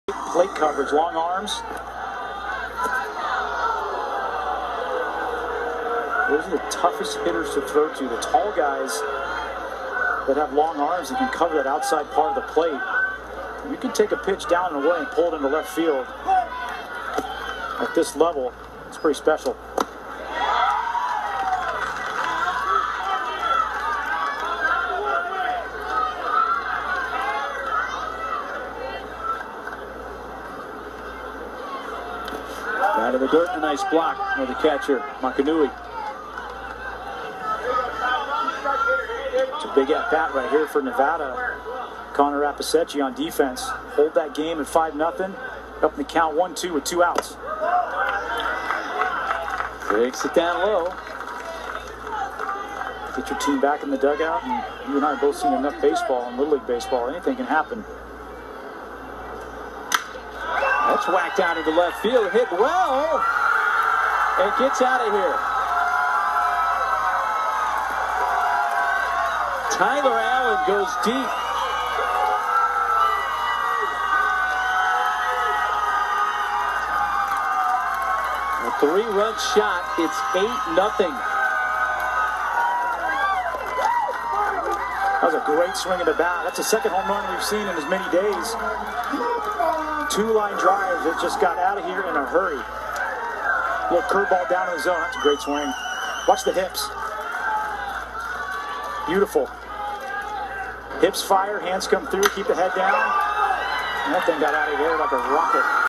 Tyler Allen 3 Run Home Run - 2018 LLWS California vs Nevada
Crushed that Sucker